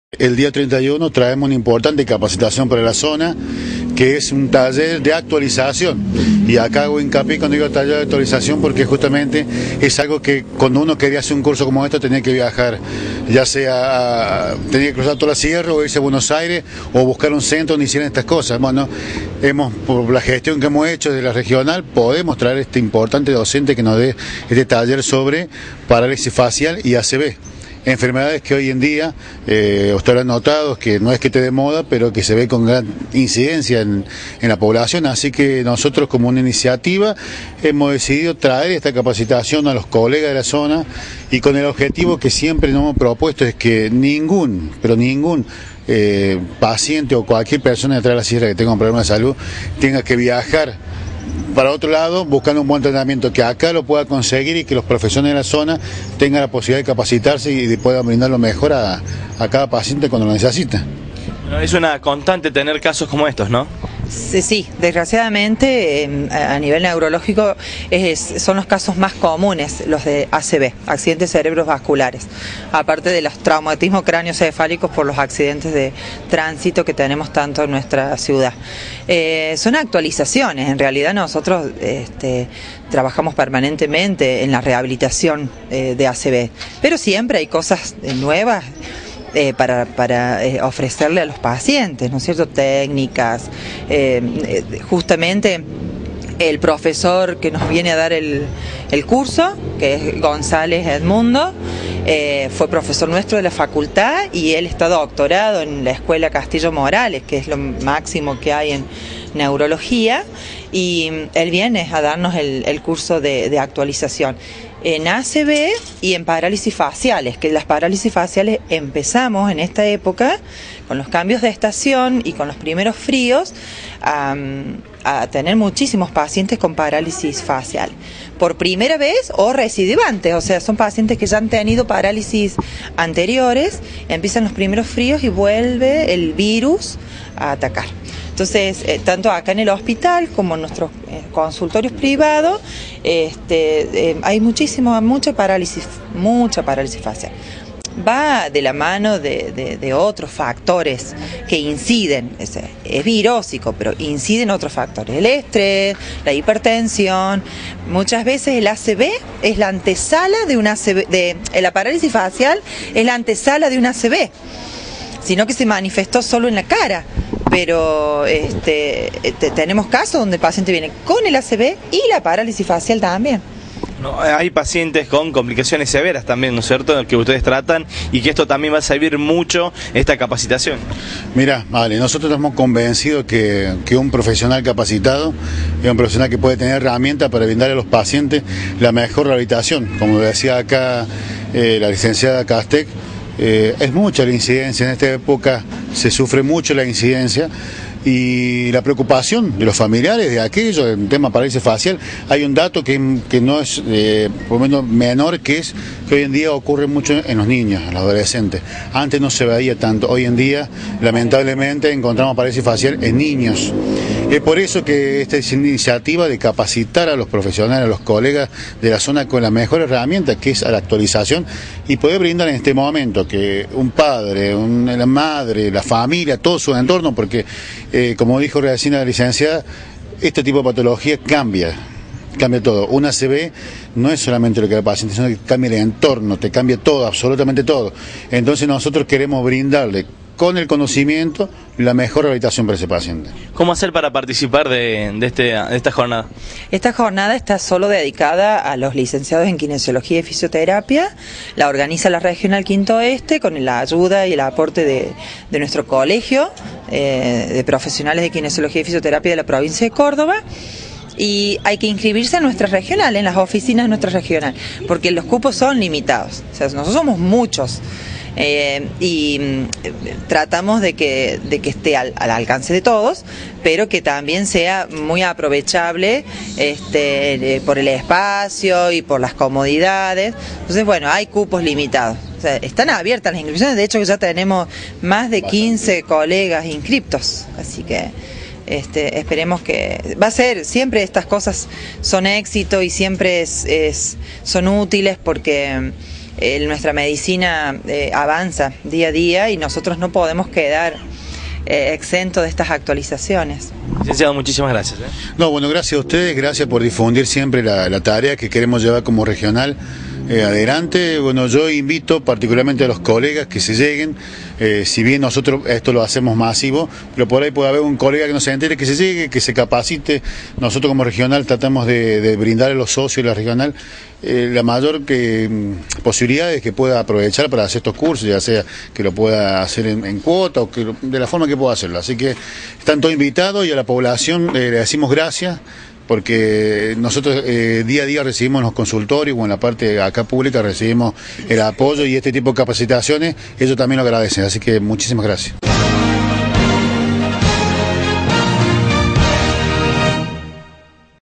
JORNADA SOBRE PARÁLISIS FACIAL Y ACB
Con el Presidente del Colegio de Kinesiólogos Regional V Lic. Francisco Acosta y la Lic. Gabriela Castex dialogamos sobre lo que será esta jornada.